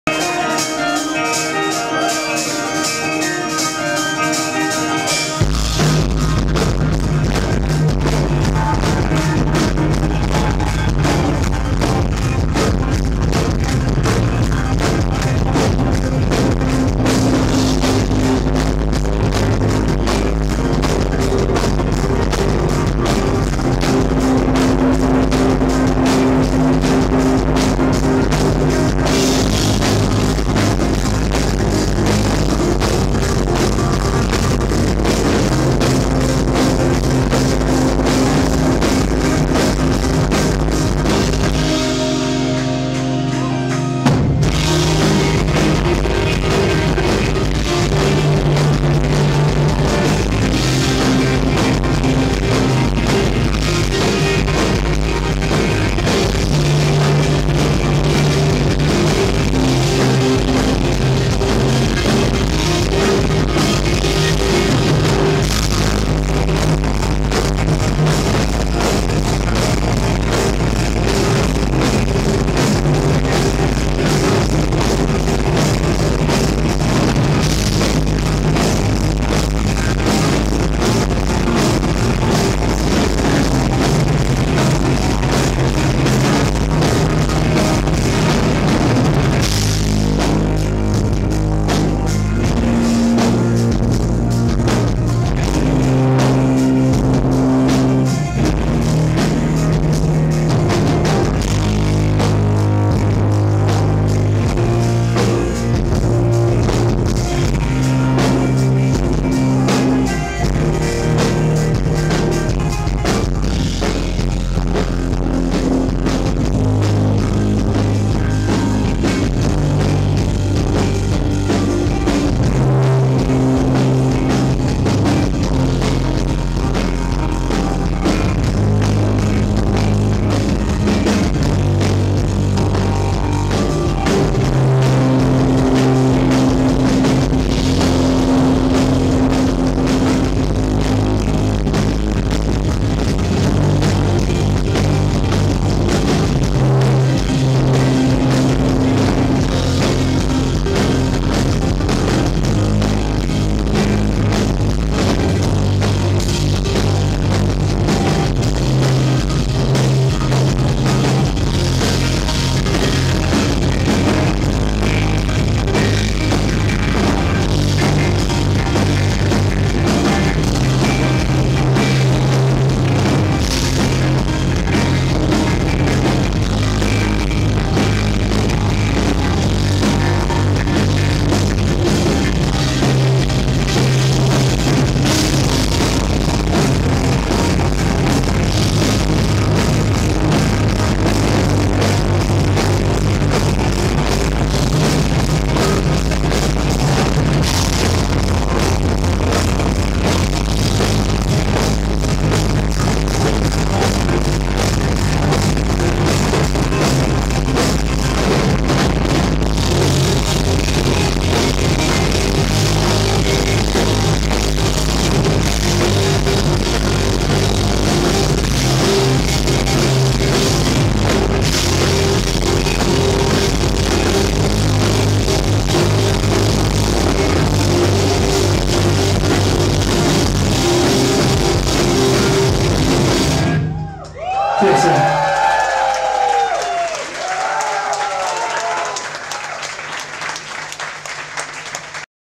Maybeshewill live in Hong Kong(@Hidden Agenda) - 8/15
I'm very sorry about the sound quality, just try to record for friends to feel their live here in HK